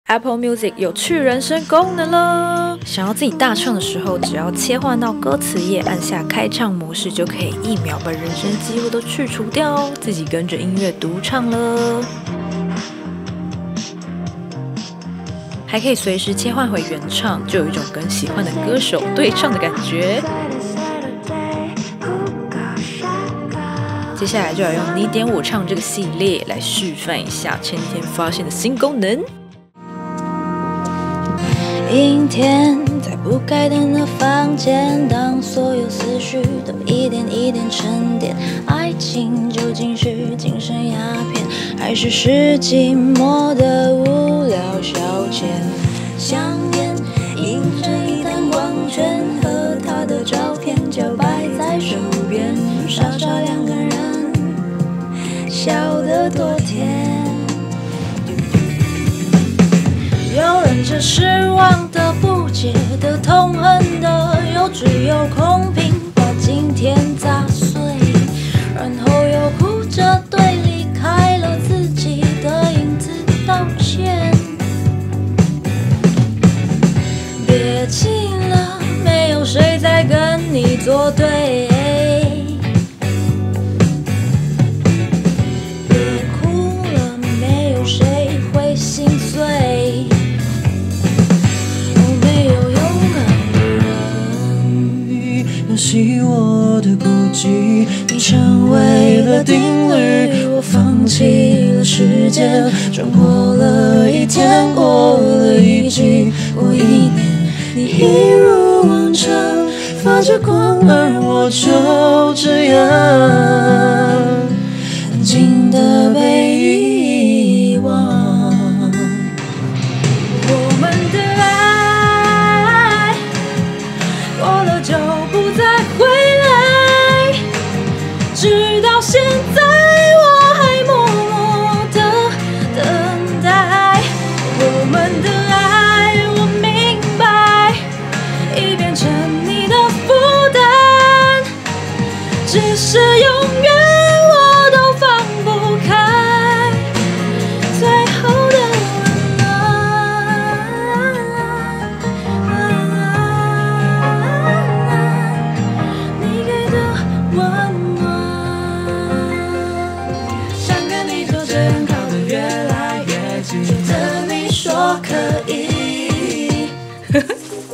Apple Music開唱新功能！內建去人聲爽唱KTV！
有人也是 AppleMusic 派嗎！
上禮拜發現的開唱新功能🤣
查了一下才發現2022年12月就出了哈哈哈哈（撥接網路？）
對我這種愛隨地哼歌的傢伙來說實在太讚ㄌ
（最後一首在鬧a）
【本影片沒有開啟營利】